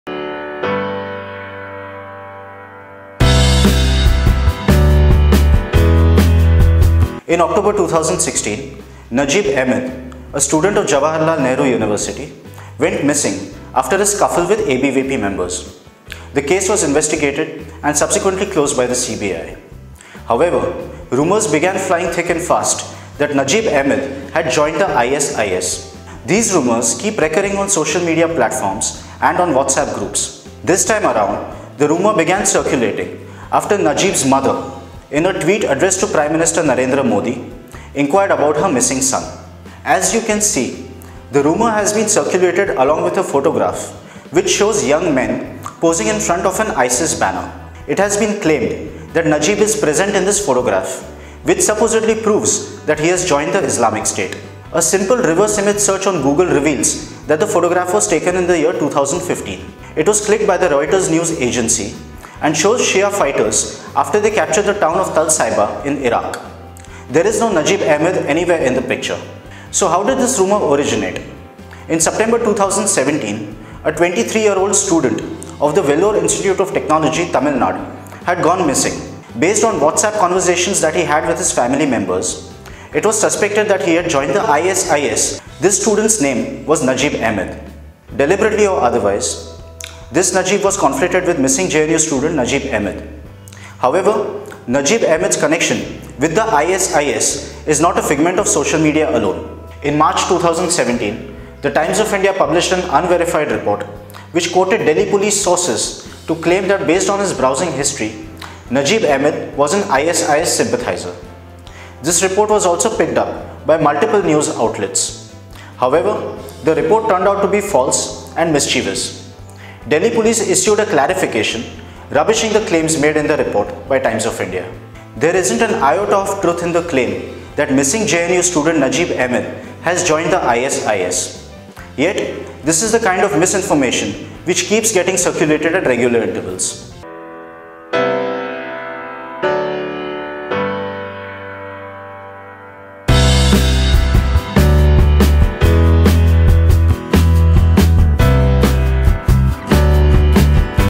Missing JNU Student Najeeb Ahmed has joined ISIS? False claim
Misinformation that refuses to die. An image that becomes viral every few months with the false claim that missing JNU Student Najeeb Ahmed has joined ISIS is circulating yet again. Watch this and share the fact-check widely.